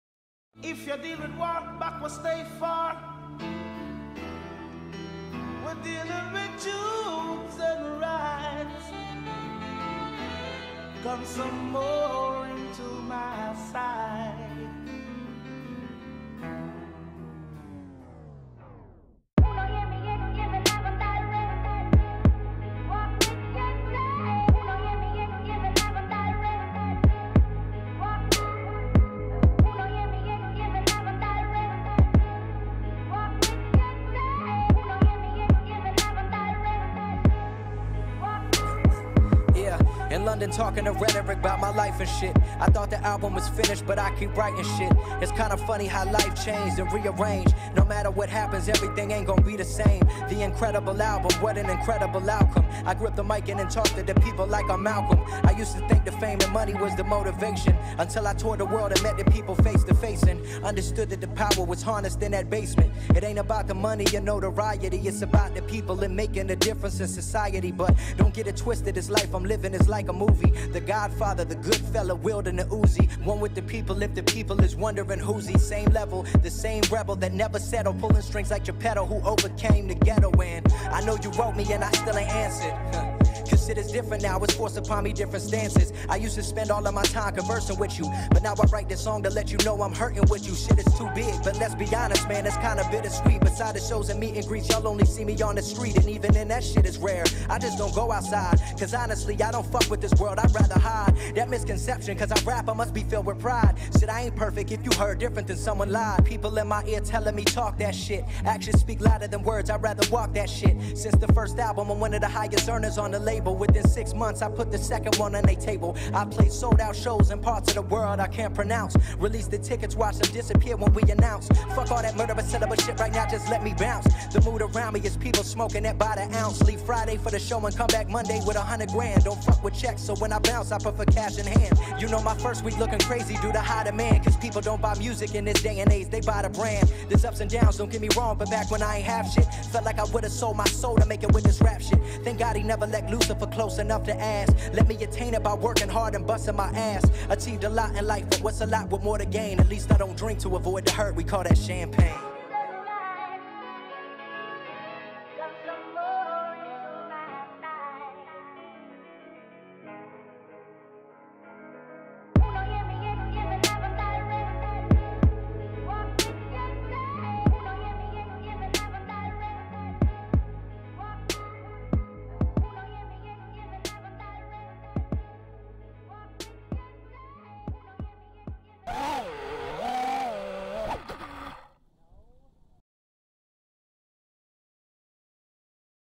FPV Drone Freestyle ICT FPV Barbecue 2019
Just some fun freestyle at our clubs Barbecue. Second time using an HD camera on my 5 Inch drone